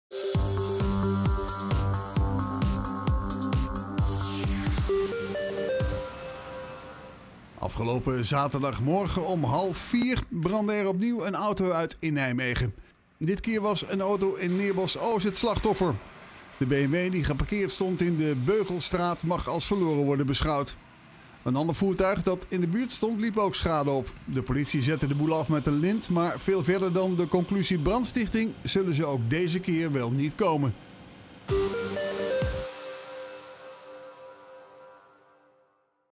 Auto brandt volledig uit bij brand in Neerbosch
De brandweer is in de nacht van vrijdag op zaterdag uitgerukt naar een autobrand. De brand vond plaats in de Bugelstraat in de Nijmeegse wijk Neerbosch.